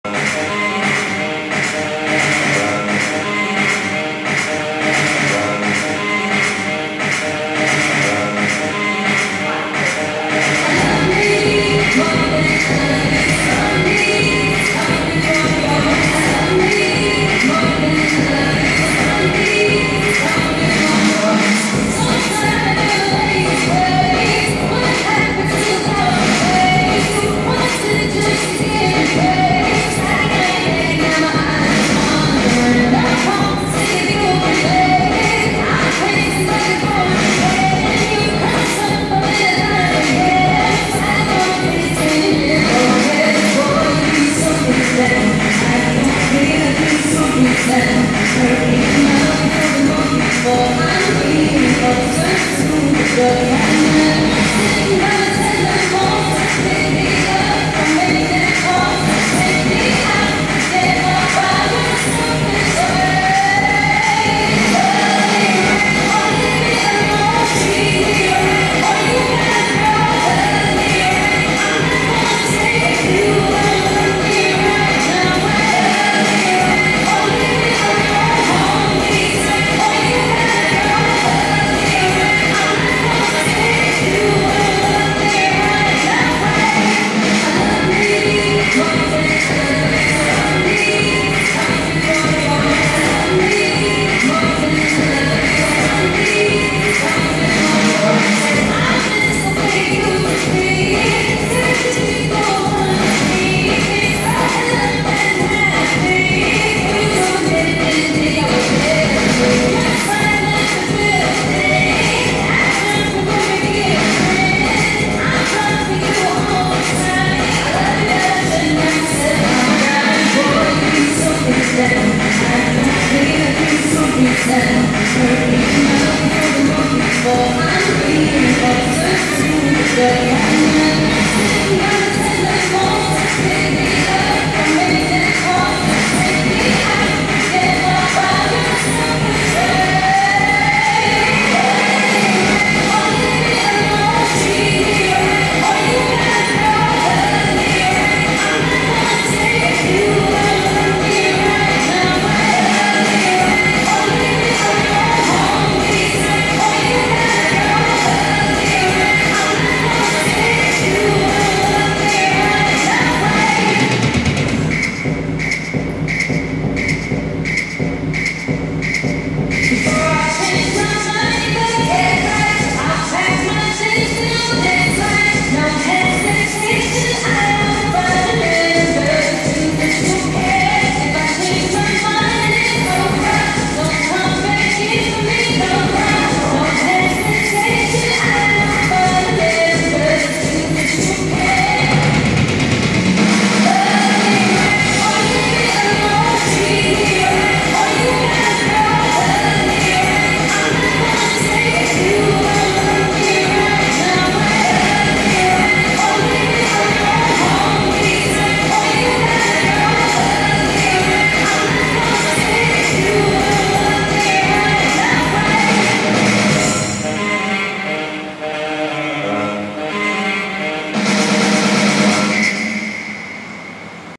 LOVE ME RIGHT line dance (Rachael McEnaney UK USA) & (Amy Glass USA) Nov 2013
Choreographed by Rachael McEnaney, UK/USA) & Amy Glass (USA) Nov 2013.
Description:  64-count 4-wall Intermediate level line dance.